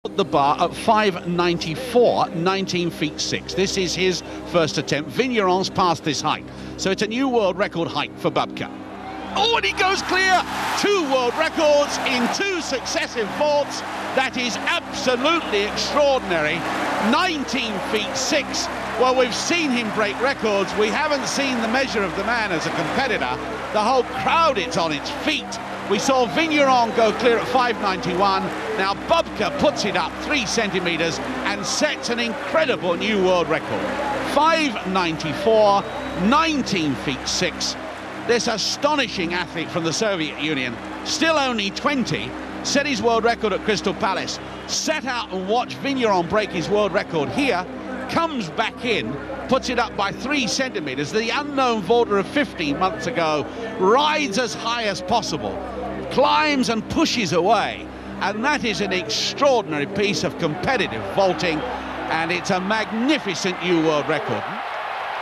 Sergey Bubka(WR)Rome,1984. Pole Vault(5.94m)
The fourth of the remarkable Ukrainians 17 outdoor world records.What made this rather special was that he had just lost his WR in the previous round to Frenchman Thierry Vigneron with a 5.91 vault. Bubka showed his competitive streak with a new mark of 5.94 on the very next jump!!